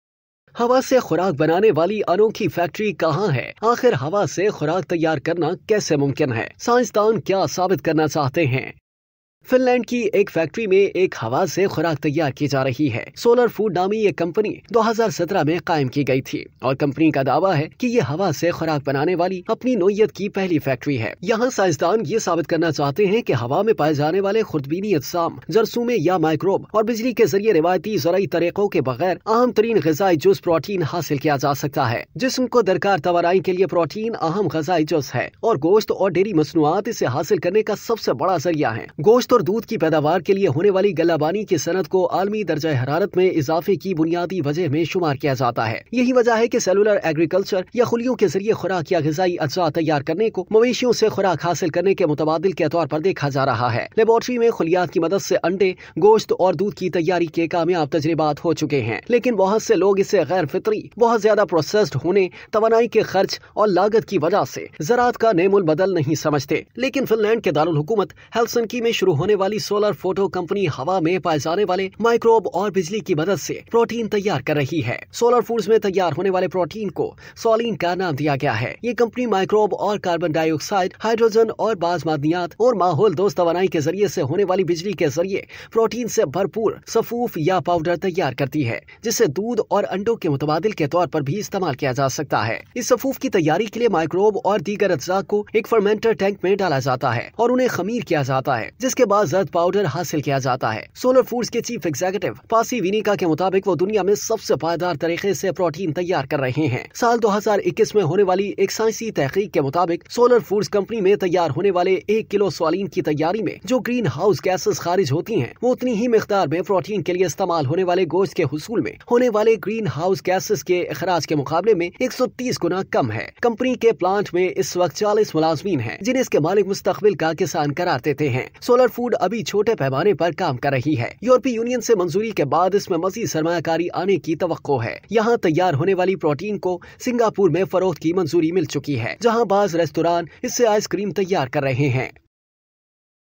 ہوا سے خوراک تیار بنانے والی انوکھی فیکٹری کہاں ہے؟ | Food From Air | Unique Factory
A Finnish company, Solar Foods, is pioneering the production of protein from air, using microbes and electricity, as a sustainable alternative to traditional agriculture.